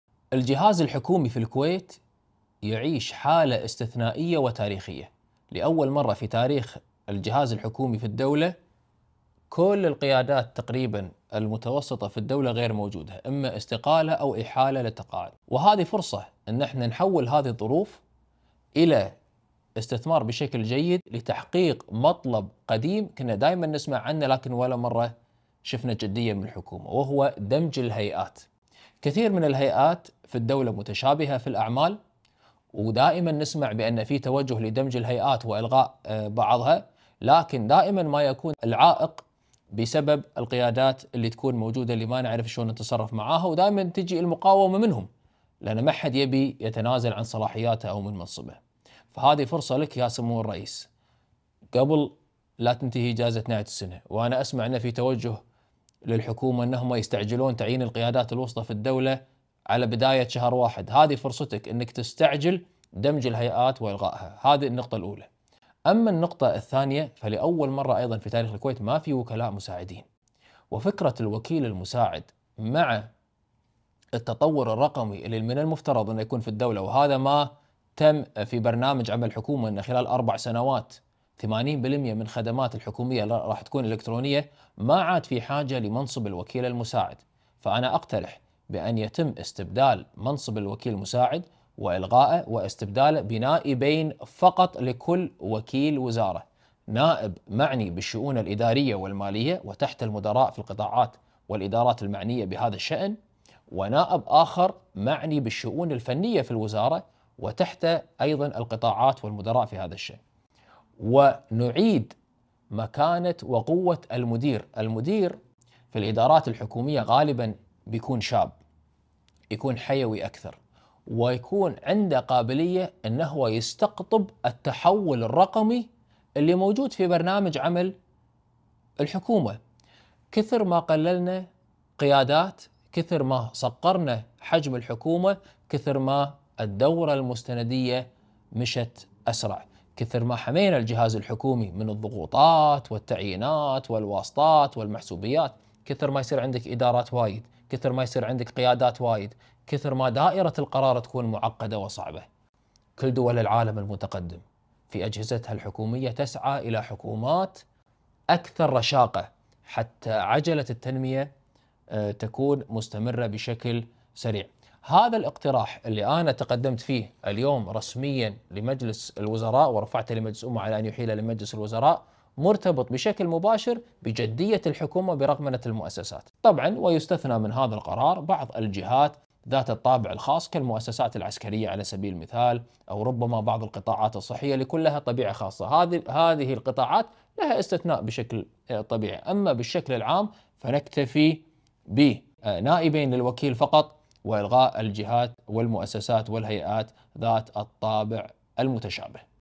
النائب عبدالوهاب العيسى لرئيس الوزراء الشيخ أحمد النواف: أمامك فرصة ذهبية في فراغ القيادة الوسطى
بأن تستعجل إلغاء هيئات ودمج غيرها تحت كيانات جديدة